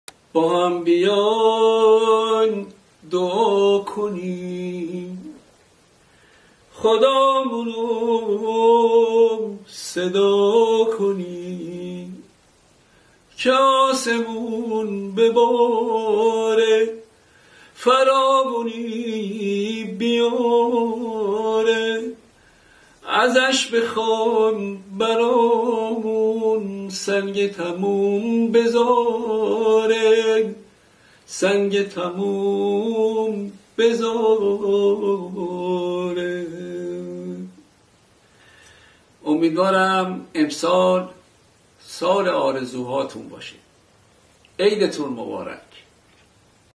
پیام تبریک نوروز 1396 استاد معین
پیام تبریک نوروز 1396 استادمعین به همه مردم ایران